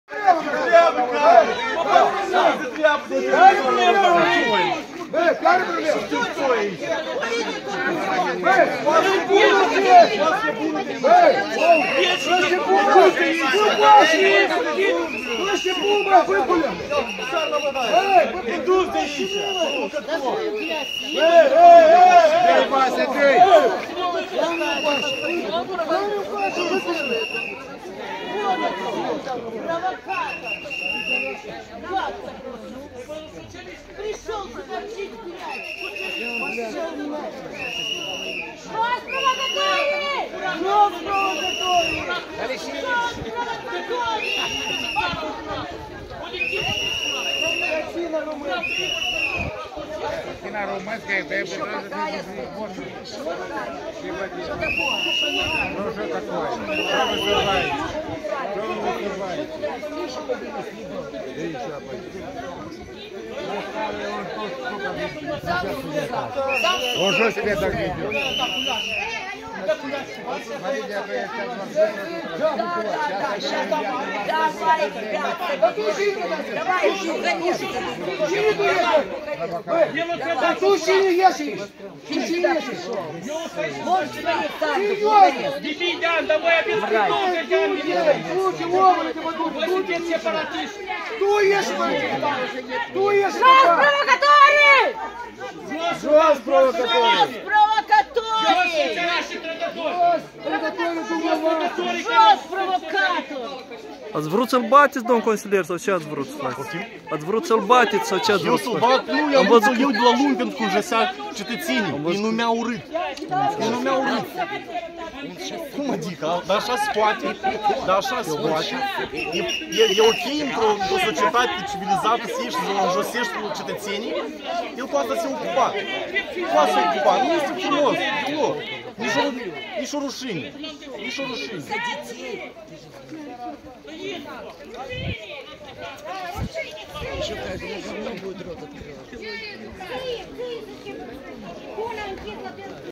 Incident în fața casei lui Igor Dodon
Incident în fața casei fostului președinte socialist al R. Moldova Igor Dodon, acolo unde mai bine de șase ore au loc percheziții. Mai mulți oameni, printre care și un consilier municipal socialist, s-au luat la harță din motiv că susținătorii lui Dodon „ar fi fost numiți cu cuvinte urâte”.

Principalii protagoniști ai scenei din fața casei lui Dodon sunt consilierul municipal socialist Victor Poleacov și Vasile Sinigur, bărbatul care la un protest al veteranilor de război, desfășurat în 2020 în fața Guvernului, a spart una dintre ușile clădirii Executivului cu un buzdugan.